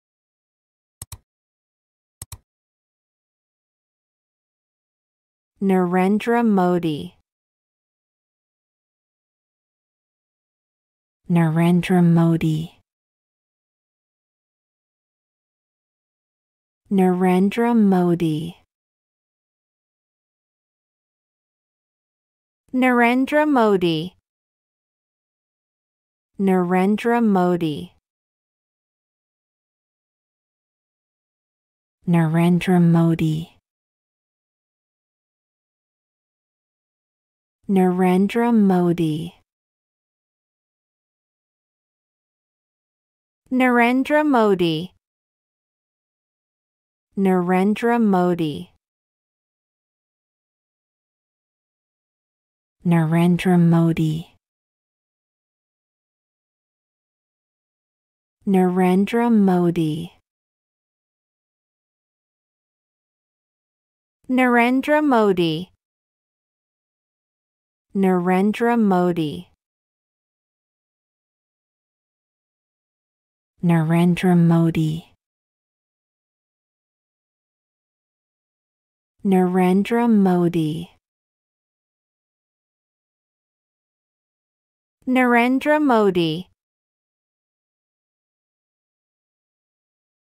How to Say Narendra Modi [American Accent]
How to say Narendra Modi. Listen to the Narendra Modi audio pronunciation in English.

Narendra Damodardas Modi is an Indian politician who has served as the 14th prime minister of India since May 2014. Modi was the Chief Minister of Gujarat from 2001 to 2014 and is the Member of Parliament for Varanasi. Wikipedia
Born: September 17, 1950 (age 72 years), Vadnagar, India